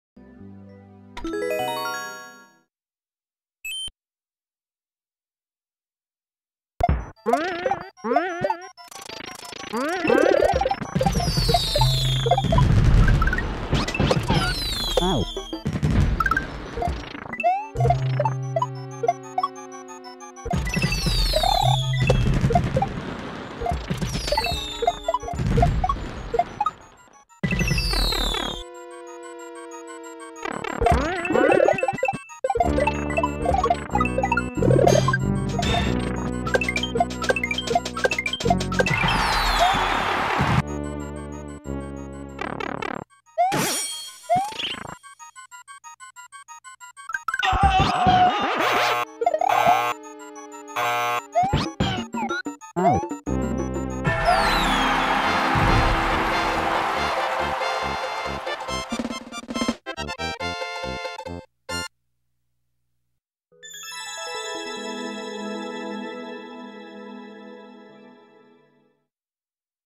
Dado's Mid~Machine TeamMidair (by Jay⇔Kaizo)
Awesome TeamMidair level from jay kaizo. I one shot everything yoshi ditch surfer.